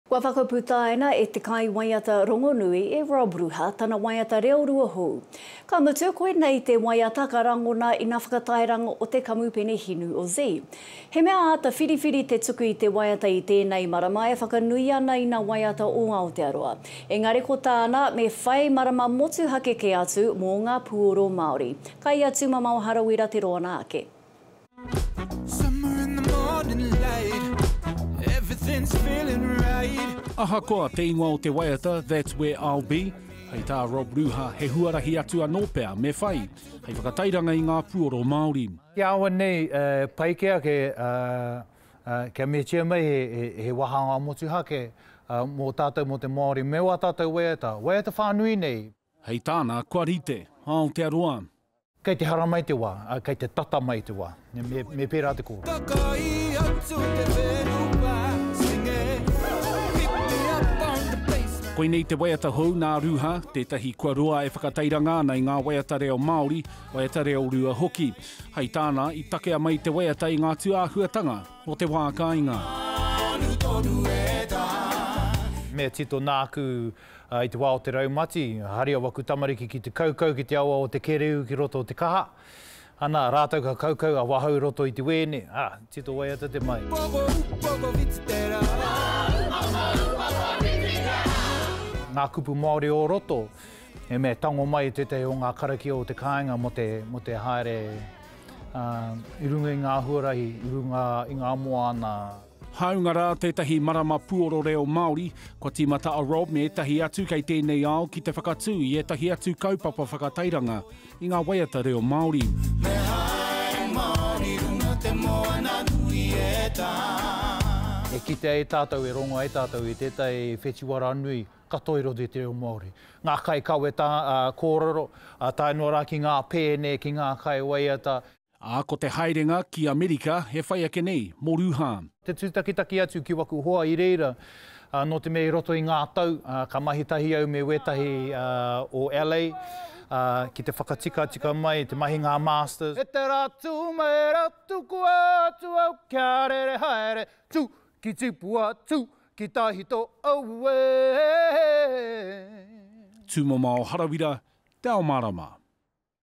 Rob Ruha releases new song, calls for Māori music month | Te Ao Mārama
Popular musician Rob Ruha has a new bi-lingual single that's also being used to promote fuel company Z. But how is Māori language music being served by New Zealand Music Month?